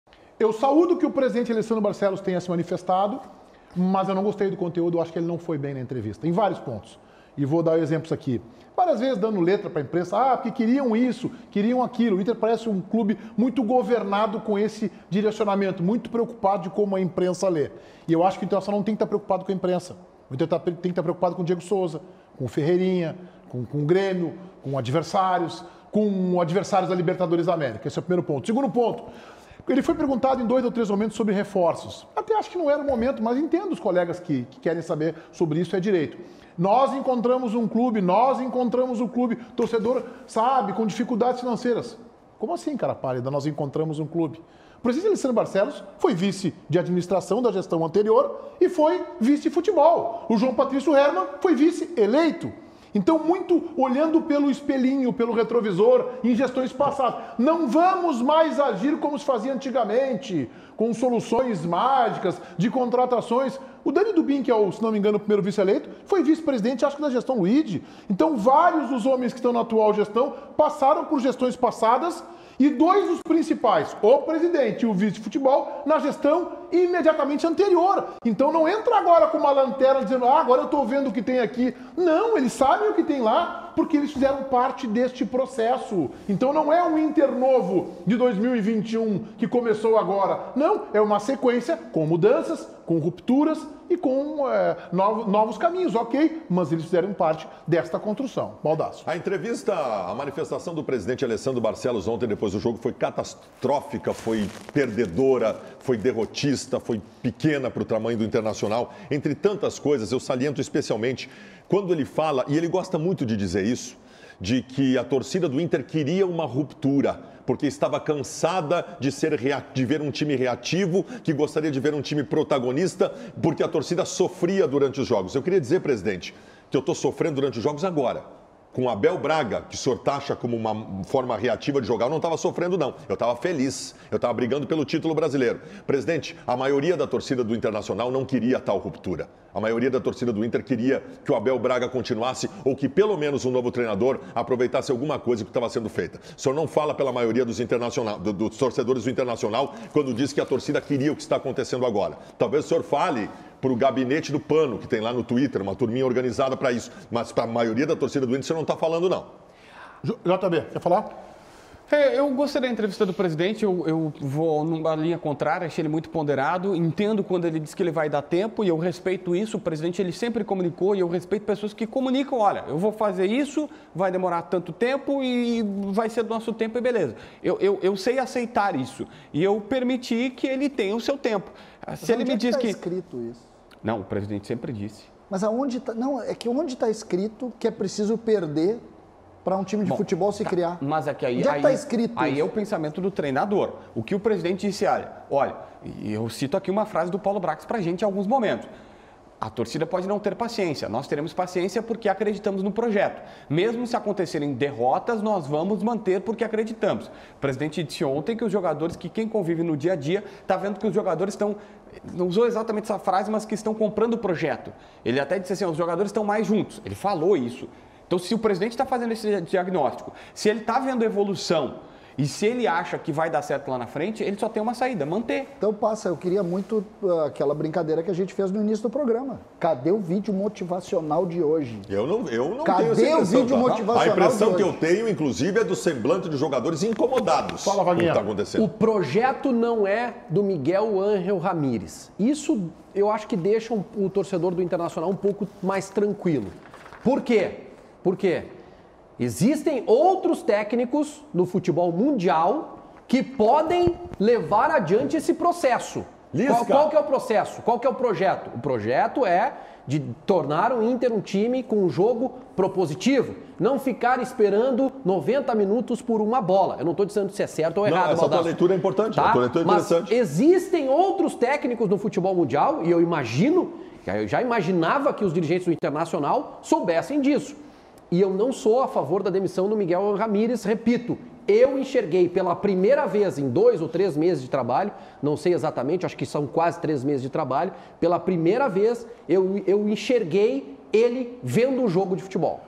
Baldasso manda recado a presidente do Inter: "Tô sofrendo agora, com o esquema do Abel eu era feliz"
Gostou do vídeo? Aqui você encontra cortes como esse, além dos programas completos de segunda a sexta, a partir das 12h30!